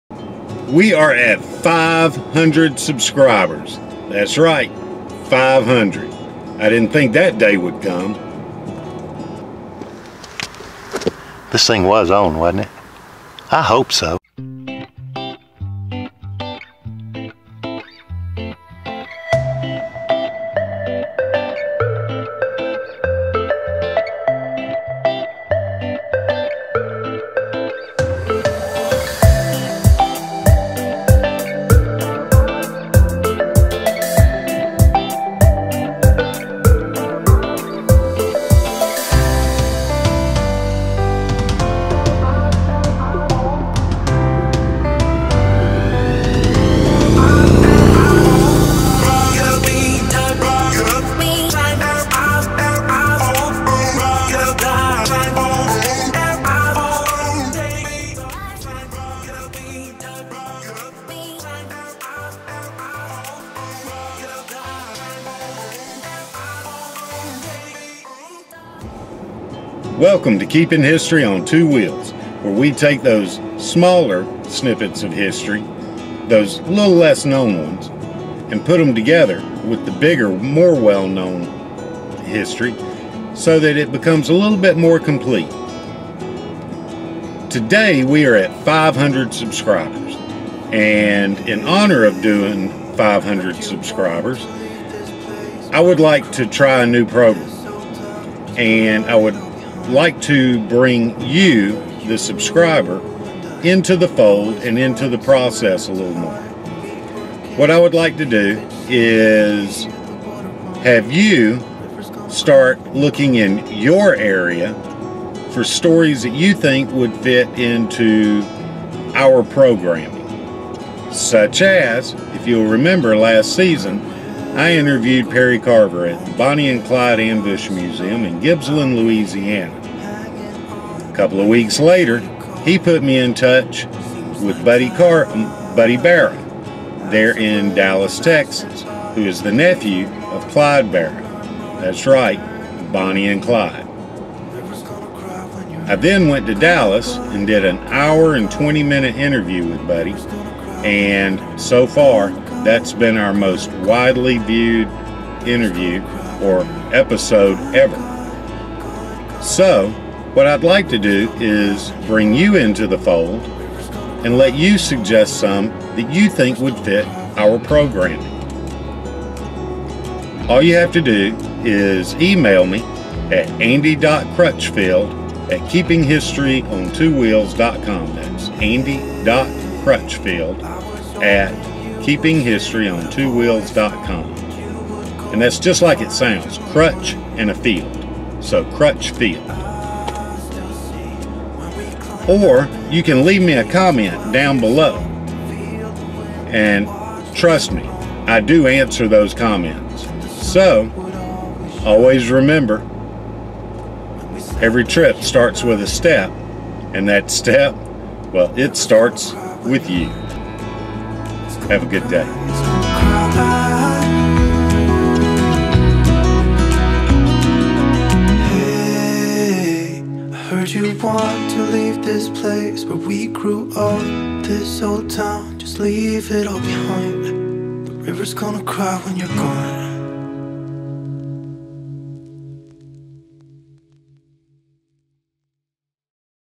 The 500 and YOU
And in honor of that, we are changing up how we do this to include you. Leave us a comment of a story in your area that you would like to see done. If it fits our channel we just may do it. And in helping Keeping History on Two Wheels we may put you in front of the camera for that episode. This is all a part of helping make this channel a group effort and instill ownership in the channel. So lets see what you got.